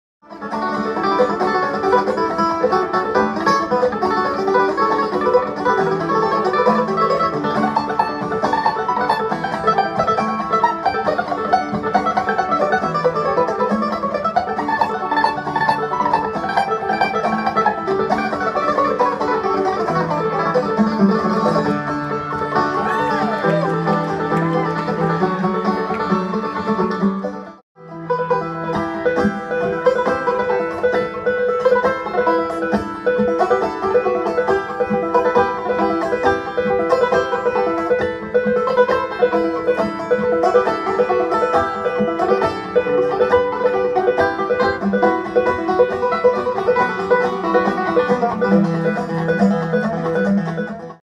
Bela Fleck and Abigail Washburn @ Telluride 2015
Bela Fleck and Abi Washburn perform at Telluride 2015.